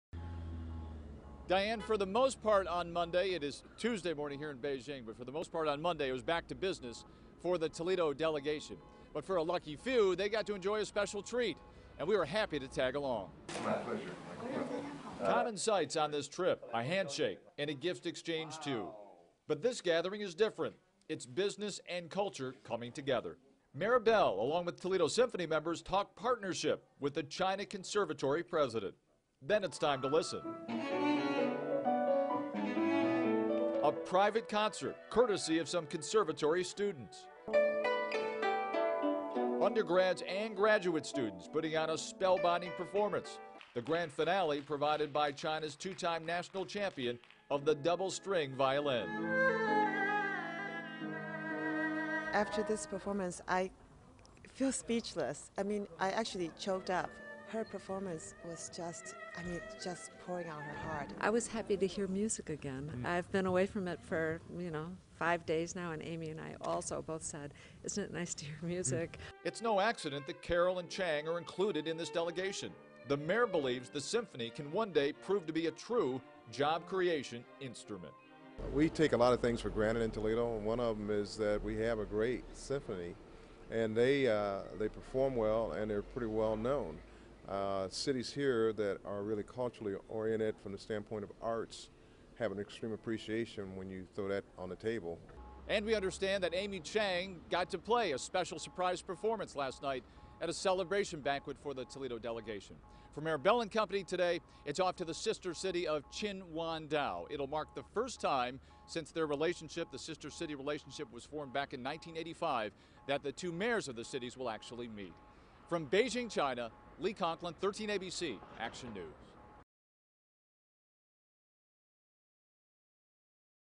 Chinese Conservatory Story
Toledo, like so many other mid-size Midwestern cities, faces numerous economic challenges.  Among them, high unemployment, a struggling manufacturing base, and population decline.  When Michael Bell ran for Mayor, he promised a "different" approach, a global approach to creating economic development, and jobs.  That "approach" would take him halfway around the world, where he would develop business, and personal relationships with a group of Chinese investors.  With the city's economic future at stake, 13abc felt Toledoans deserved a look at this economic development push for themselves.  Anchor Lee Conklin and Photojournalist Justin Billau traveled with Bell and the Toledo Business Delegation to various cities in China, including Shenzhen, the country's fastest growing metropolis, as well as Beijing.  Conklin and Billau captured the sights and sounds of the journey, providing reports for seven consecutive days.

Originally aired in 2009.